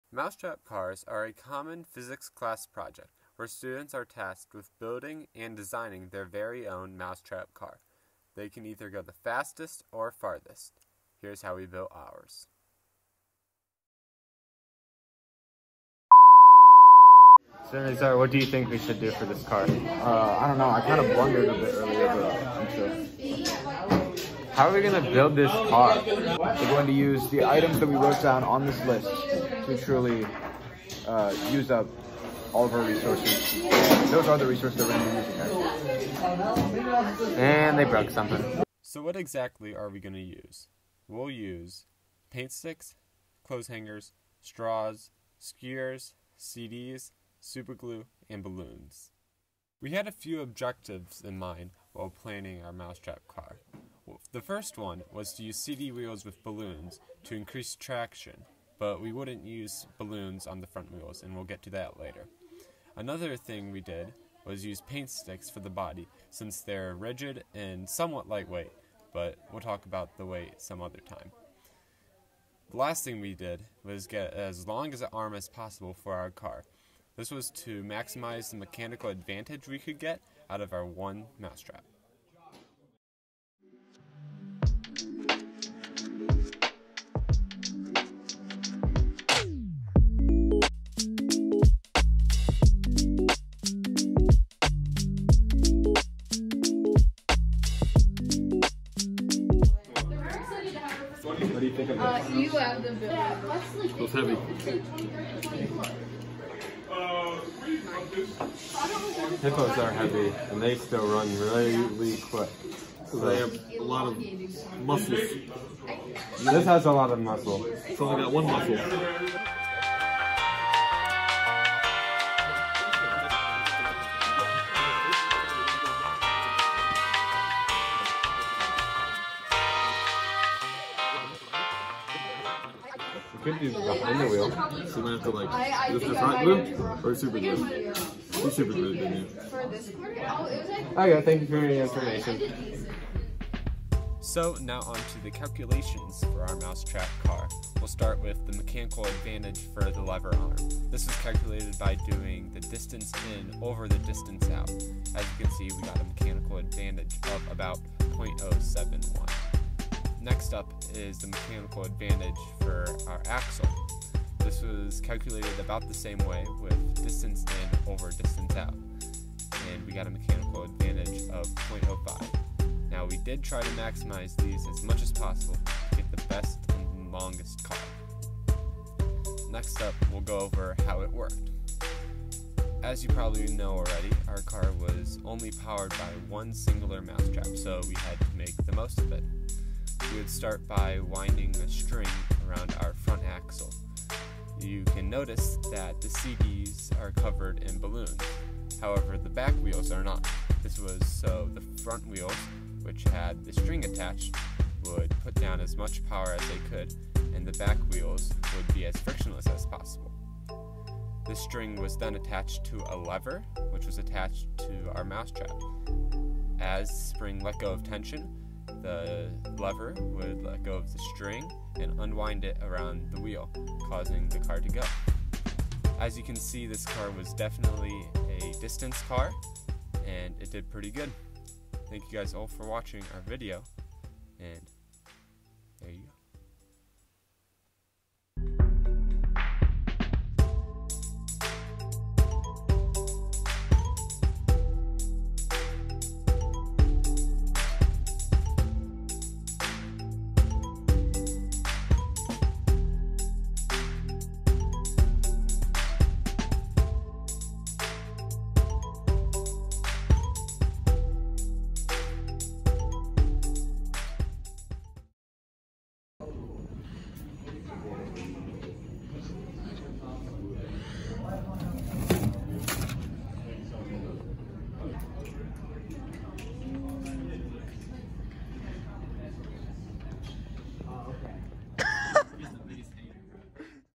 Mouse Trap Car Final Video
How we built and designed out mouse trap car.